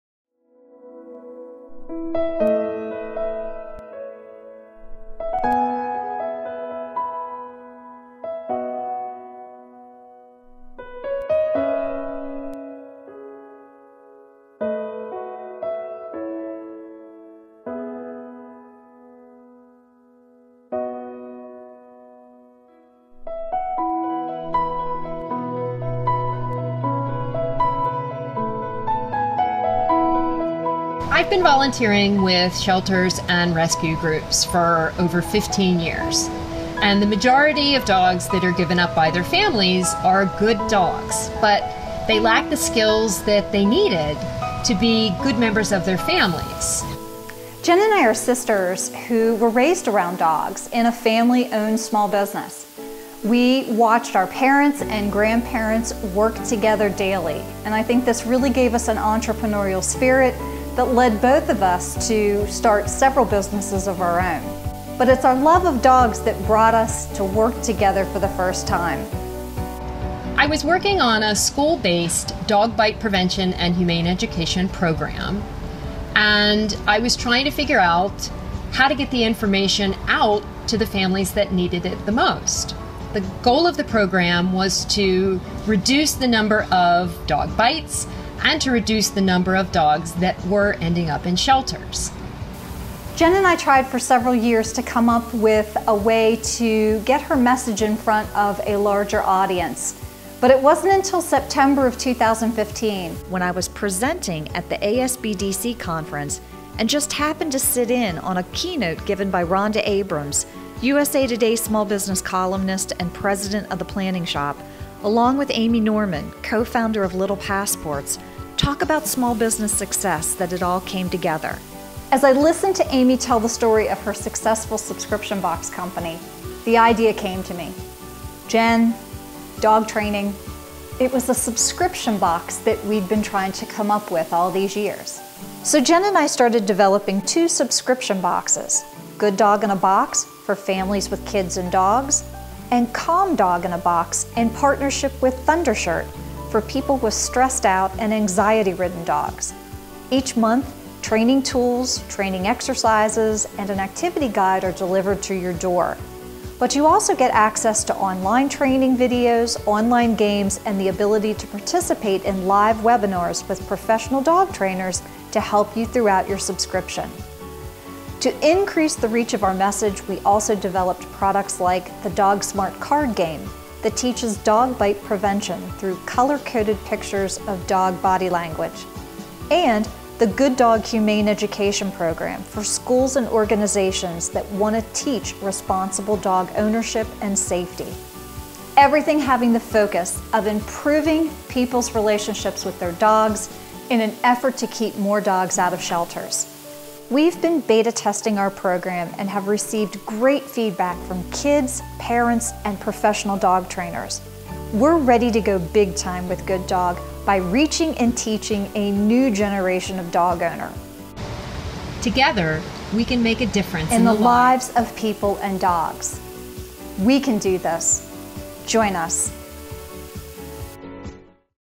How Good Dog in a Box Started
Cofounding sisters Jenn Merritt, CPDT-KA, BS, and Kim Merritt-Butler, tell the story of how they started Good Dog in a Box. Learn how the Merritt sisters are on a mission to save family dogs from being surrendered to shelters.